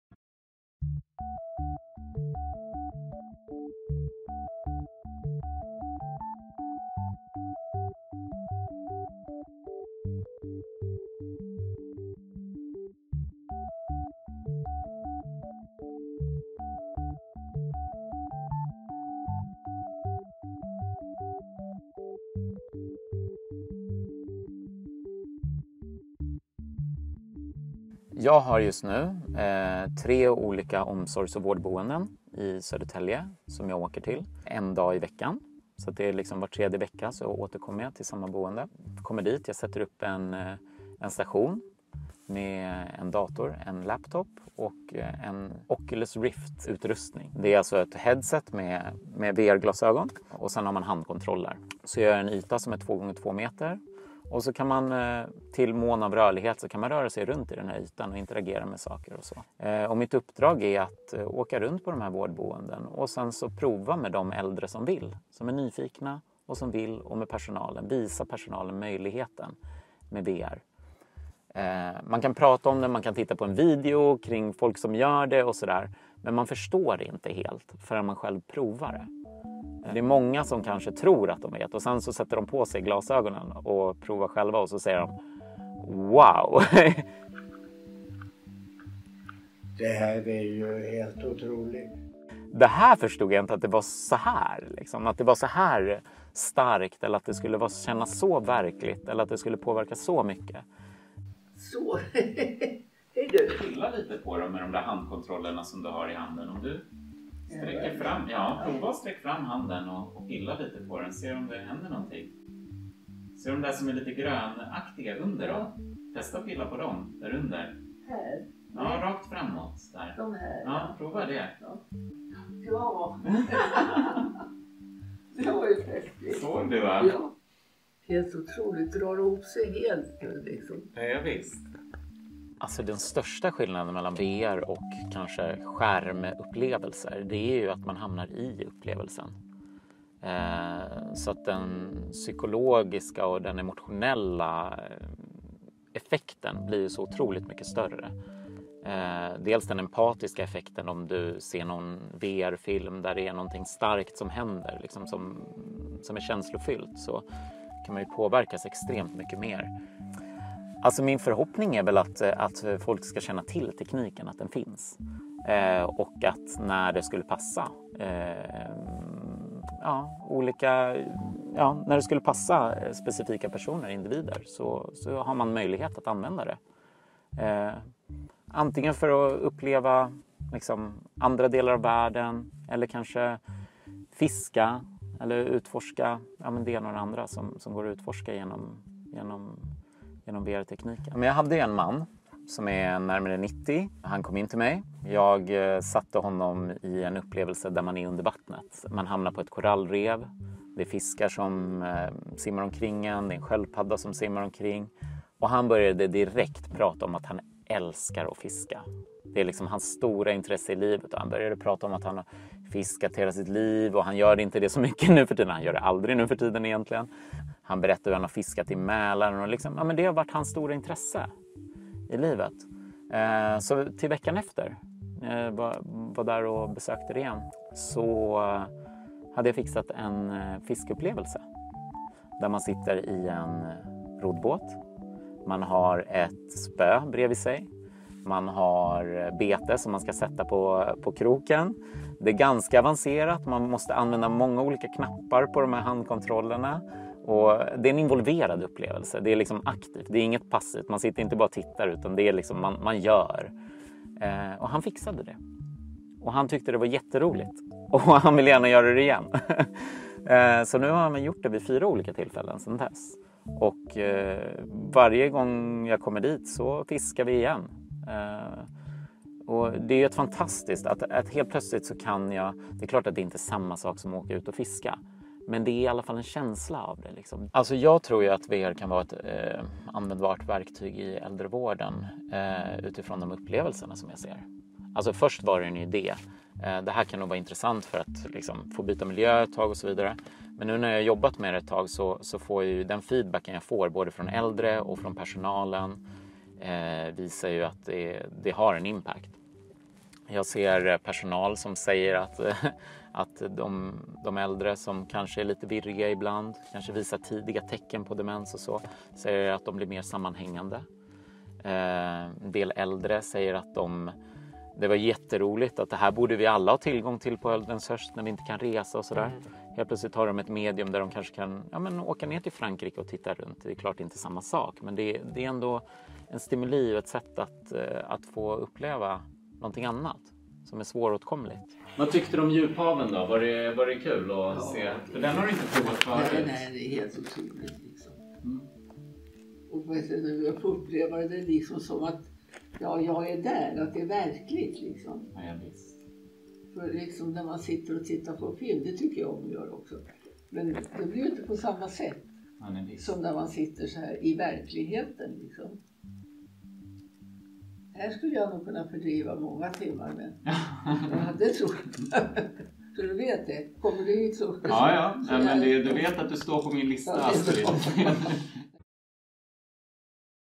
VR i äldrevården: Ett reportage med Daniel Kemppi
Daniel Kemppi är VR-pedagog och mottagare av Kulturnämndens projektstöd Kultur & Hälsa. Han använder virtual reality som redskap för att aktivera och engagera brukare på tre äldreboenden i Södertälje.

Produktion: Timmy Forss för Immersivt.se

Samt vår stora guide till VR i vården, inklusive äldrevård, psykiatri, rehabilitering och mycket mer: